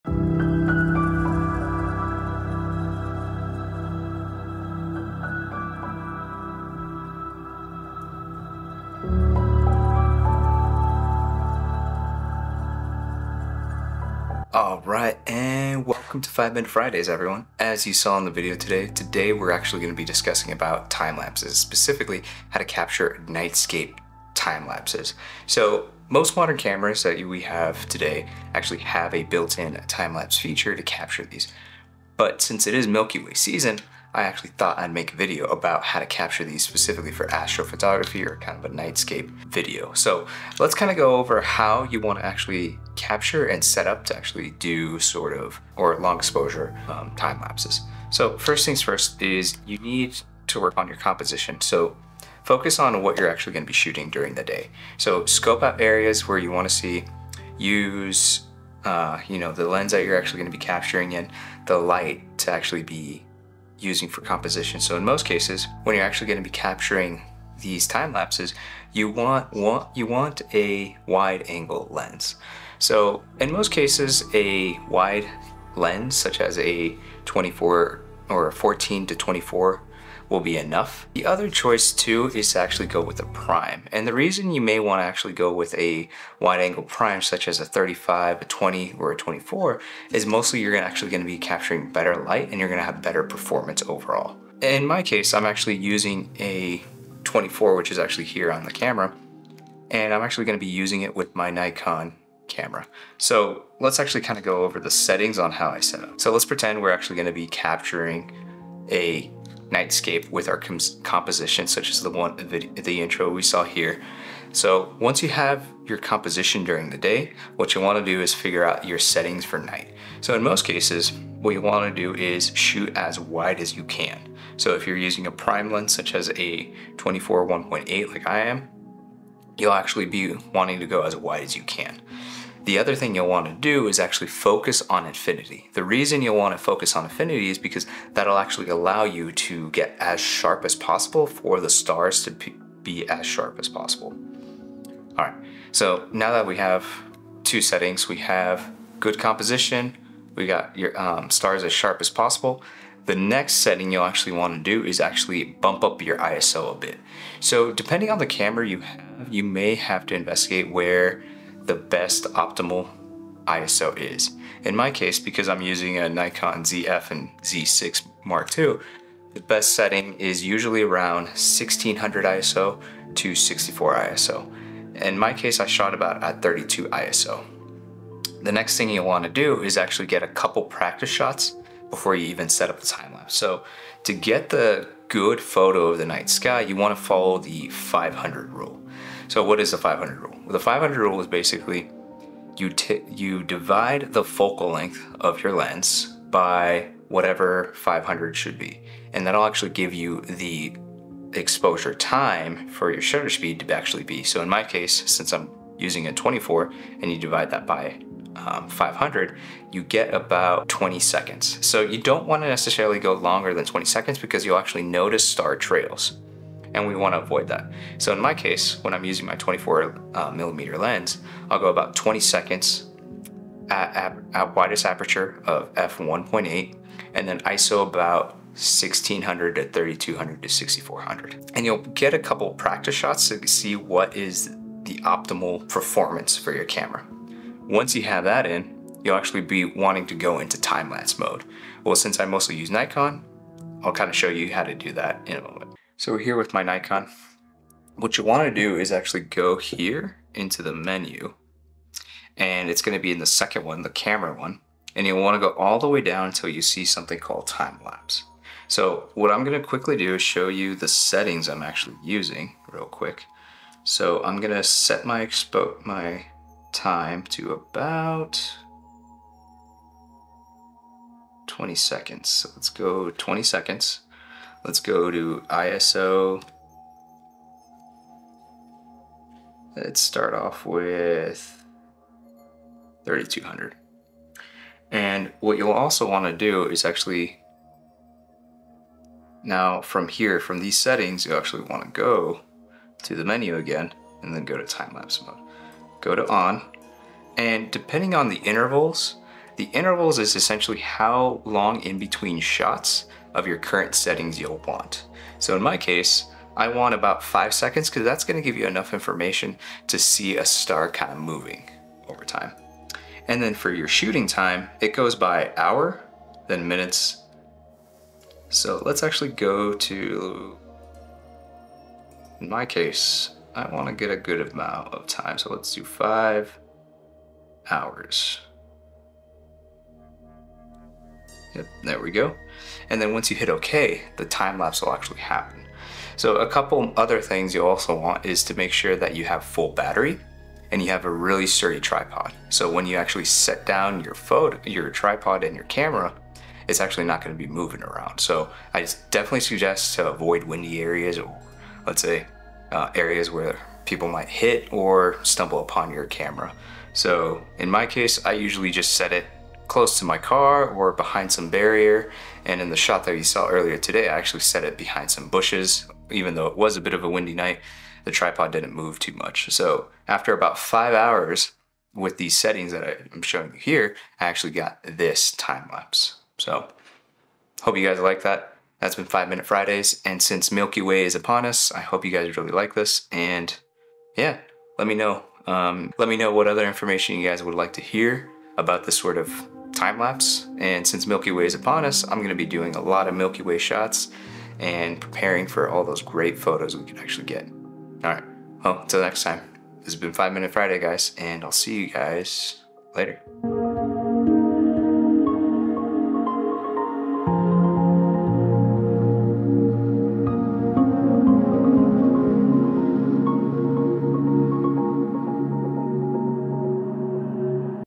Capturing Stunning Nightscape Timelapses: How to
In this 5 Minute Fridays we go over how to capture stunning astro photography timelapses. All in preparation for milky way season.